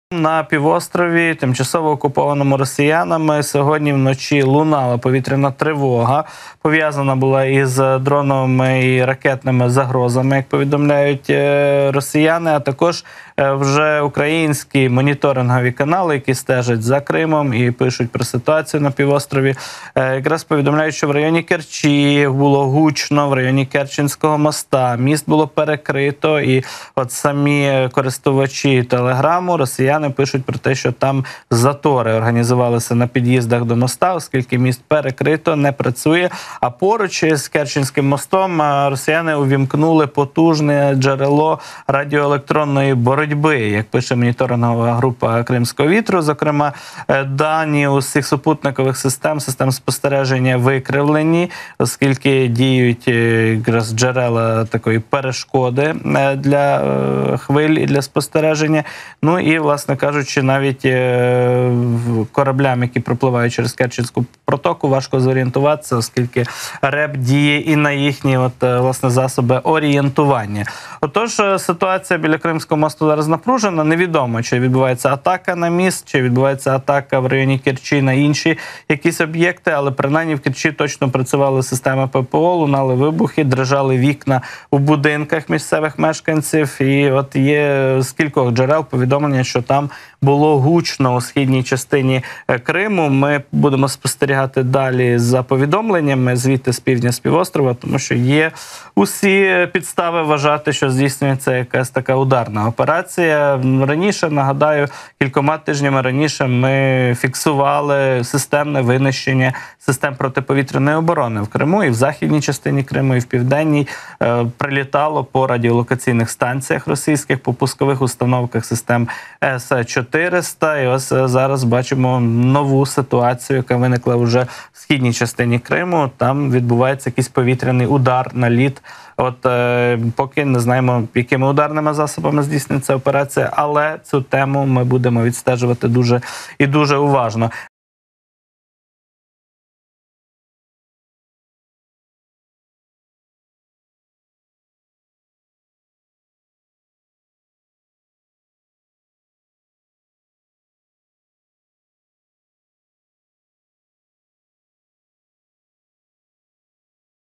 🔥 Кримський МІСТ екстрено ЗАКРИЛИ! Тривога у КРИМУ. Гучно було у ....
У Керчі місцеві повідомляють про вибухи зранку 6 грудня. За даними місцевих телеграм-каналів окупанти навіть перекрили Кримський міст. Водночас міноборони Росії вже відзвітувало про збиття. За словами очевидців, вибухи у Керчі чути ще з 05:00. Водночас бідкаються на закриття незаконно зведеного мосту. За попередньою інформацією, у Керчі гучно у районі суднобудівного заводу "Затока", пишуть, що заграву видно в Аршинцевому. Самі ж місцеві жителі скаржились і на затори, які сталися внаслідок перекриття мосту.